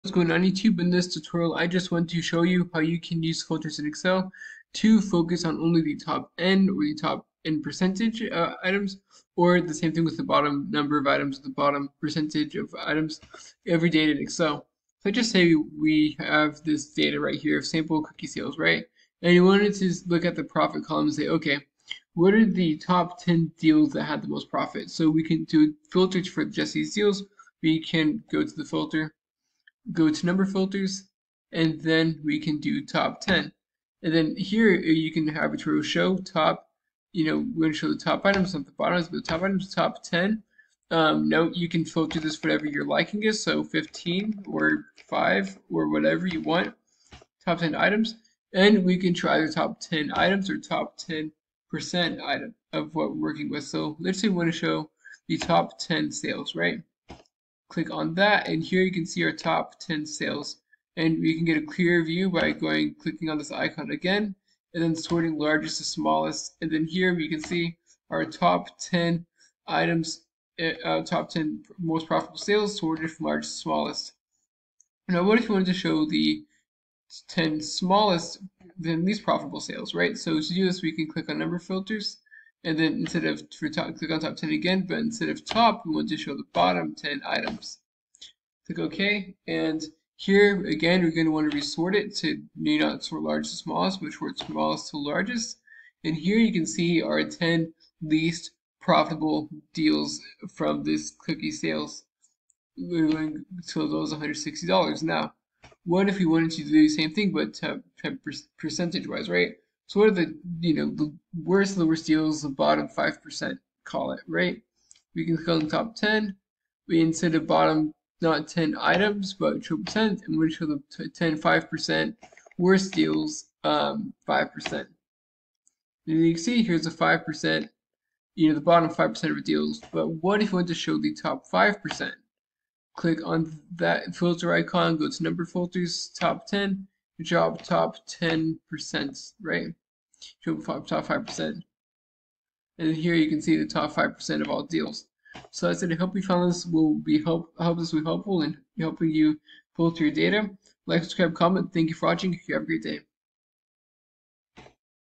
How to Filter to Just Get Top or Bottom N values, or % in Microsoft Excel
In this tutorial, I just wanted to show you how you can use numeric filters in Microsoft Excel to filter for just the top or bottom n number of values, or n% in Microsoft Excel! To do this, first turn on filters, then there is a drop down at the top where you can then choose "Top 10" after hovering over number filters in excel. After clicking on this, you can then adjust the number of items you want to filter, and if you want to filter the top or bottom values. Yu can also adjust if you want to filter for the number of items, or the % of total items.